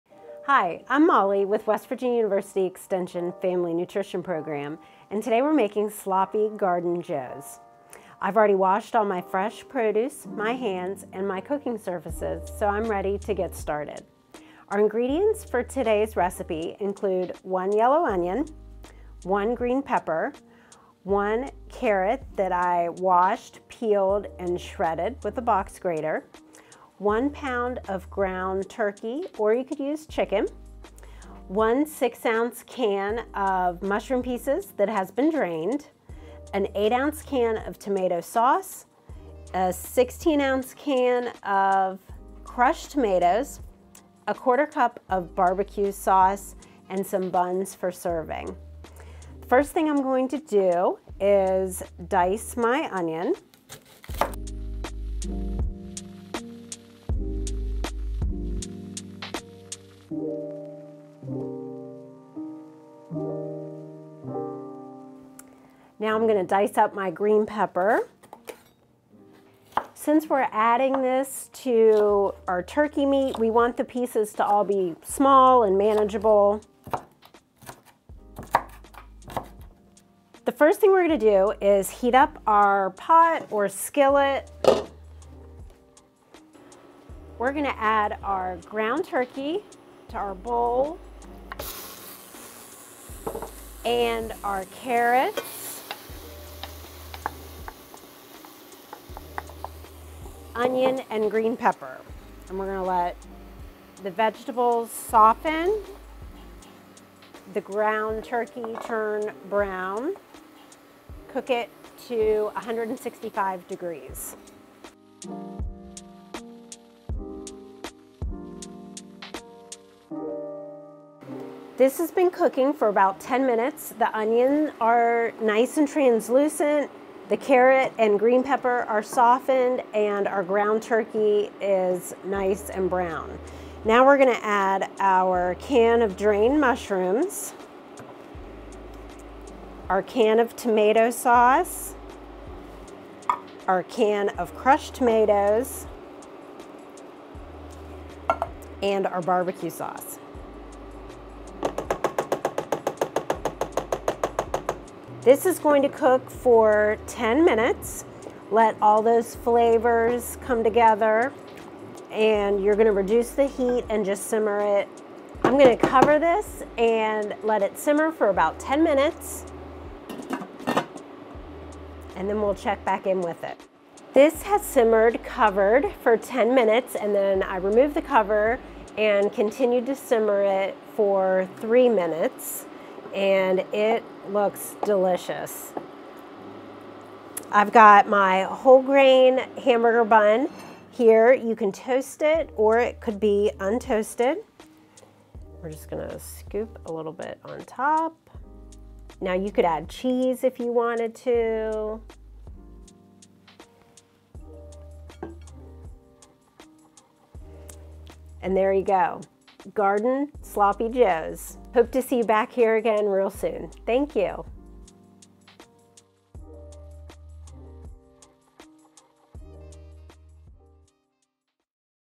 Recipe: Sloppy Garden Joes
Molly Poffenberger, health educator with the WVU Extension Family Nutrition Program, shows us how to make sloppy garden joes — a nutritious version of the lunchroom favorite.

This material funded by Supplemental Nutrition Assistance Program (SNAP) and the Expanded Food and Nutrition Education Program (EFNEP).